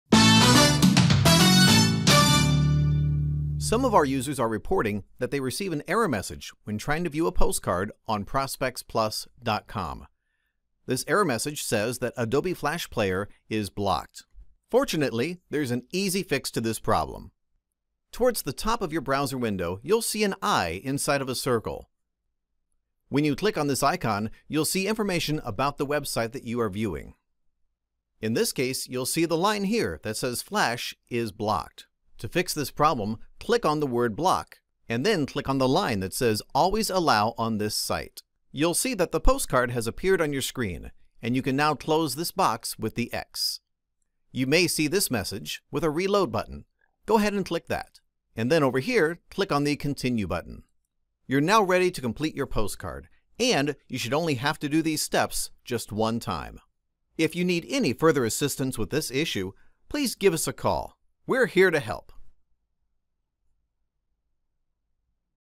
Enable Flash Player in Chrome on ProspectsPLUS.com
Some Prospectsplus.com users have had issues with Chrome blocking flash player and not allowing them to use the editor.

Follow these steps to enable flash before using the editor:

Step #1: Click on the "i" symbol to the left of the URL
Step #2: Find the flash option and click "Always allow on this site"
Step #3: Click Reload this page and continue